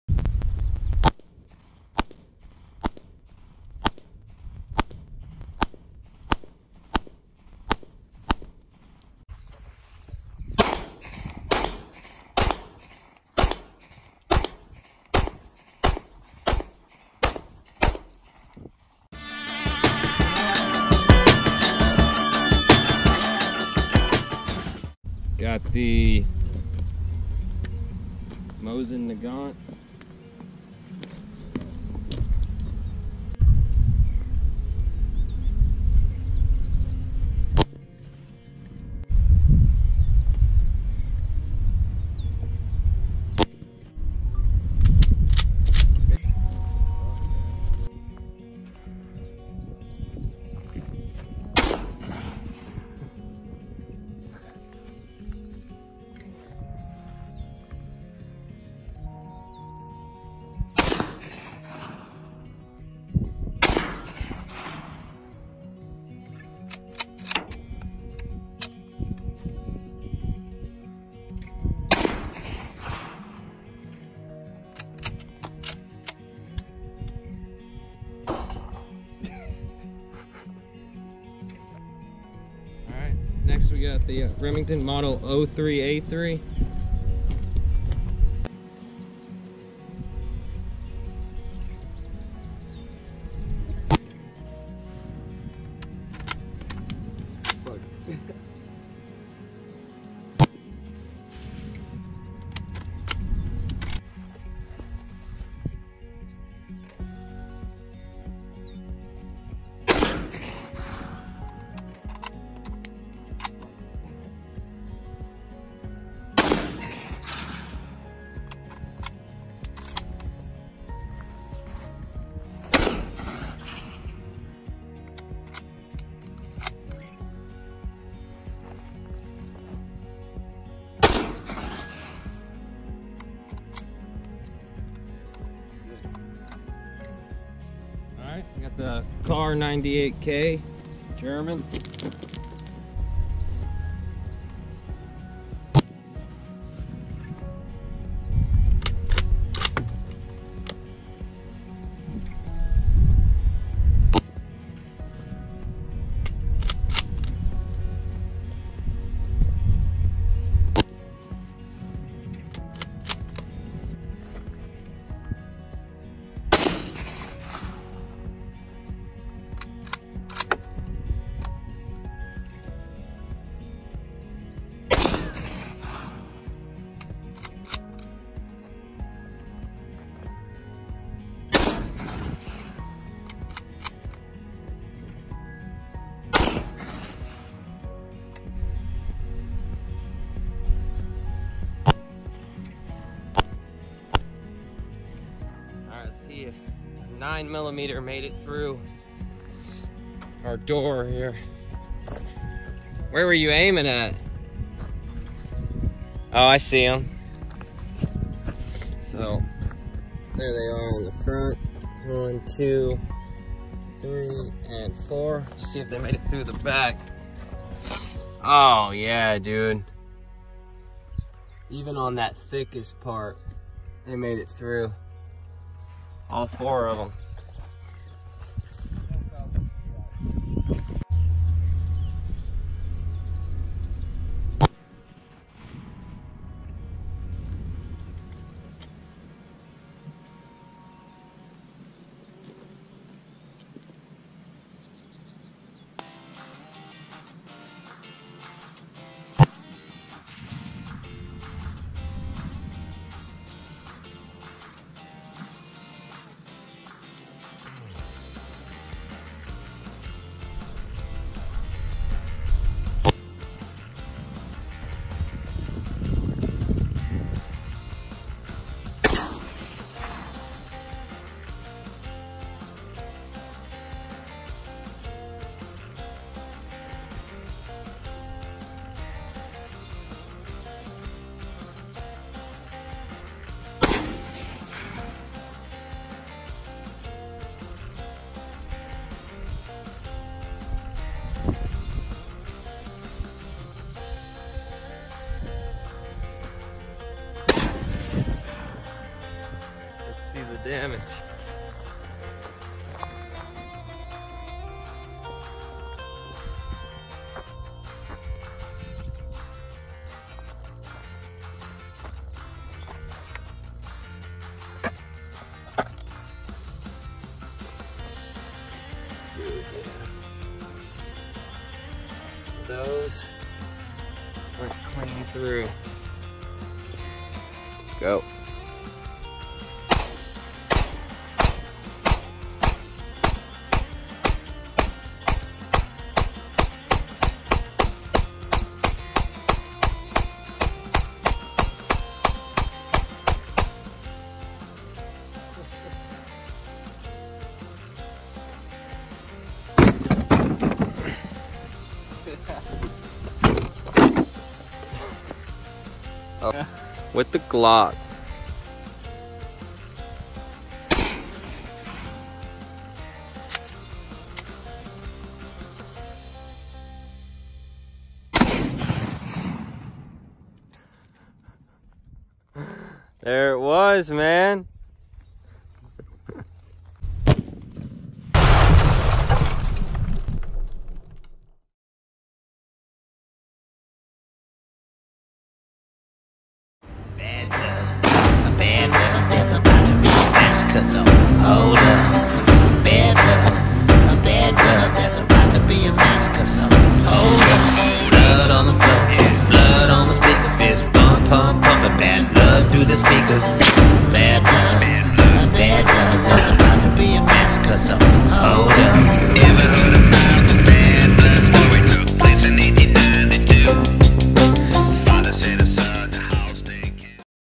Cousin shoots bolt action service rifles for the first time
Bolt Action Rifles at the Range with Caleb